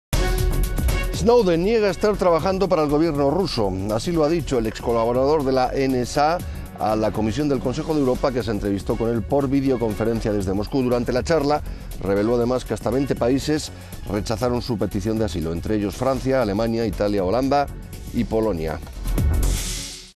Snowden niega estar trabajando para el Gobierno ruso
Así lo ha dicho el excolaborador de la NSA a la comisión del Consejo de Europa que se entrevistó con él por videoconferencia desde Moscú. Durante la charla, reveló además que hasta 20 países rechazaron su petición de asilo, entre ellos Francia, Alemania, Italia, Holanda y Polonia.